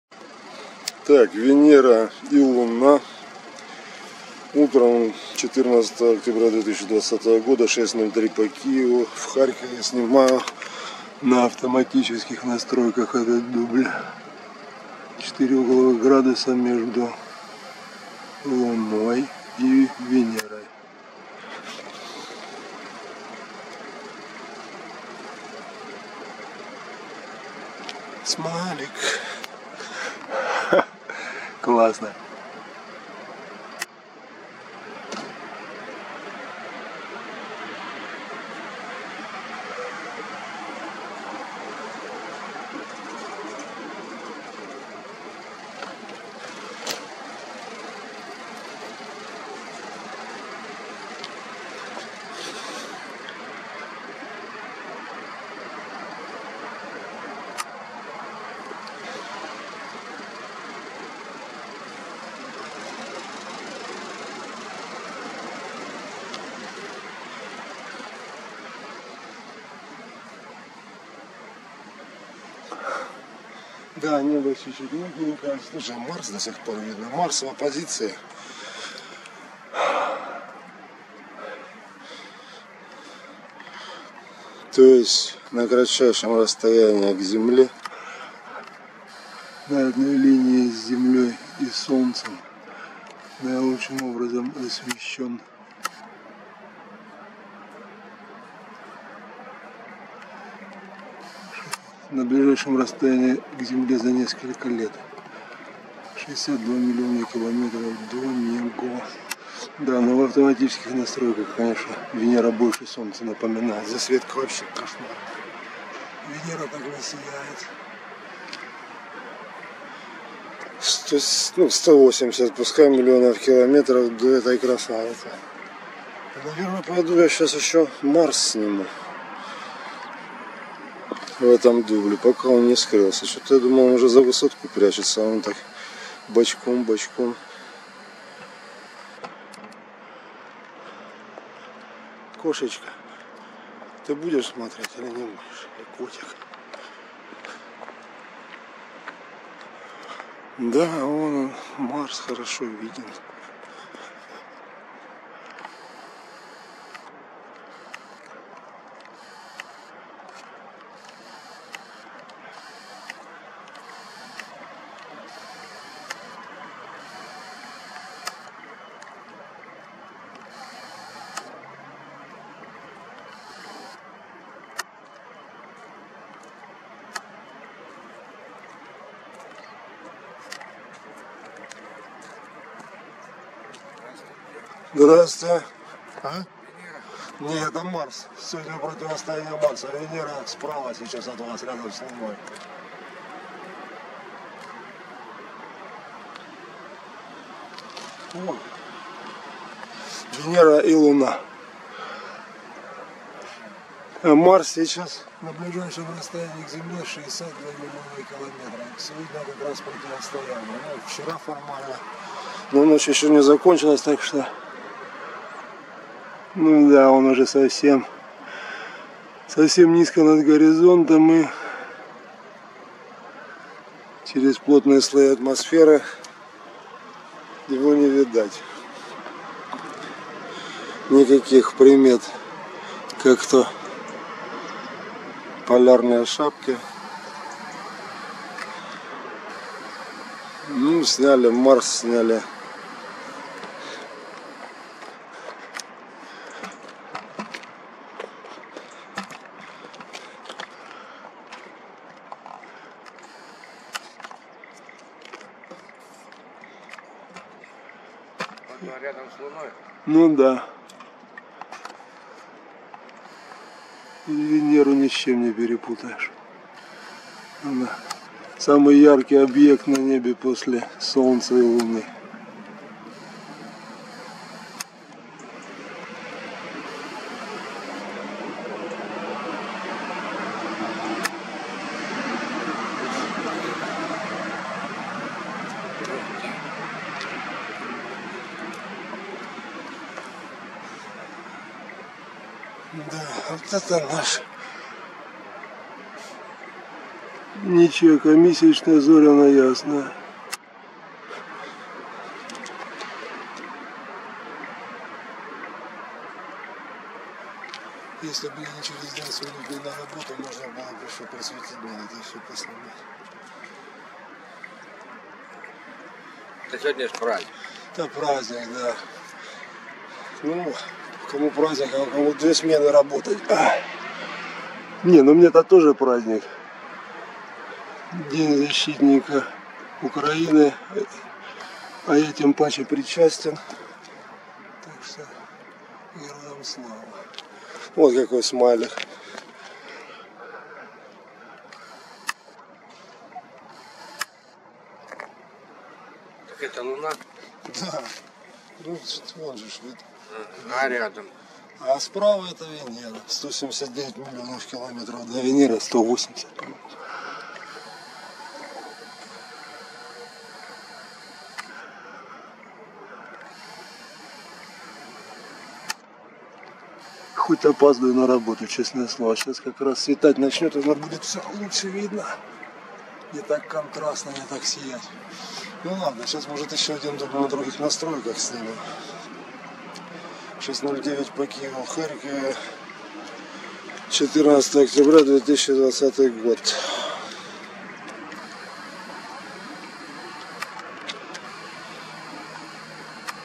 Луна рядом с Венерой 14 октября 2020 на рассвете, Марс садится за горизонт в ночь противостояния
Месяц  смайликом повис рядом с Венерой утром 14 октября 2020 года в Харькове. Марс в оппозиции садится за горизонт.